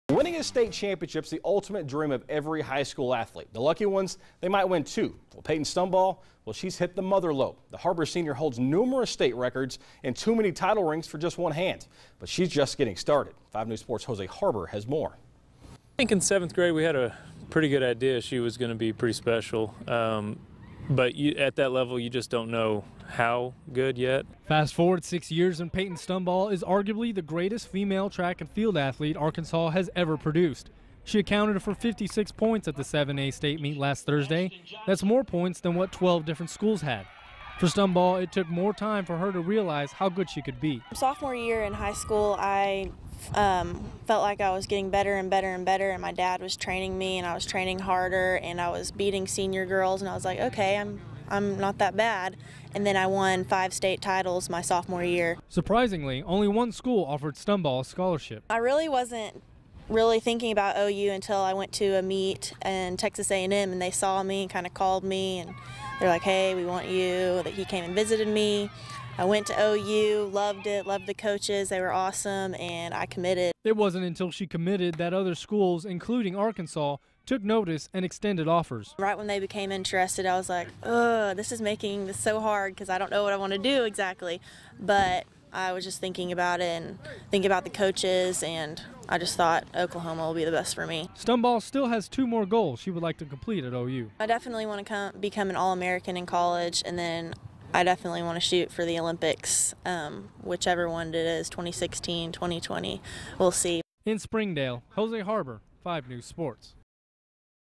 Stumbaugh Runs By Competition
Har-Ber is home to one of the state’s elite athletes. Senior Payton Stumbaugh is the fastest girl in Arkansas, a talent that her coach noticed when she was much younger.